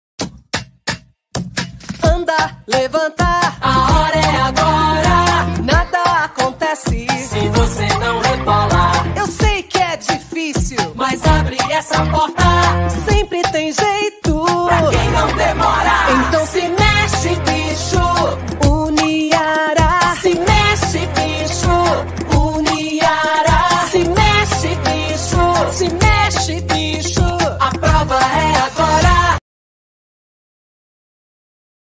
Vestibular Uniara
E aí, está achando que as coisas vão se resolver com você sentado no sofá?
Você sonha em ser bixo? Então vem pra Uniara.
Se mexe, bixo!
O mundo gira para quem corre atrás, e o melhor que você pode fazer para mudar sua realidade é se preparar para os desafios do mundo.
Comece a estudar agora! Construa sua rede de amigos, de bons professores e influenciadores, conheça gente diferente e que pensa igual a você.
Vestibular é na Uniara e a hora é agora. Se mexe, bixo!